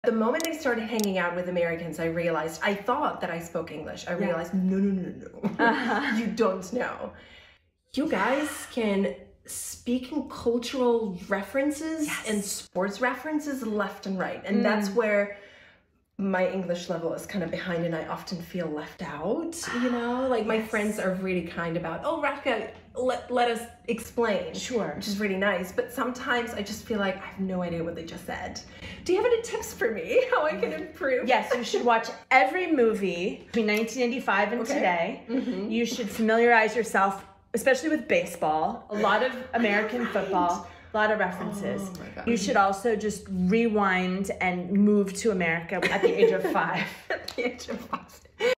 Czechs vs. Americans: How We Speak (Part 1, Part 2 is coming tomorrow)
Today I’m teaming up with Czech local @vegan.radka author of our amazing Prague for Kids and Prague for Dreamers Guides We’re talking about a fun topic — how Americans speak!

Do you like speaking in cultural references? What cultural or sports references does your language use? ⬇️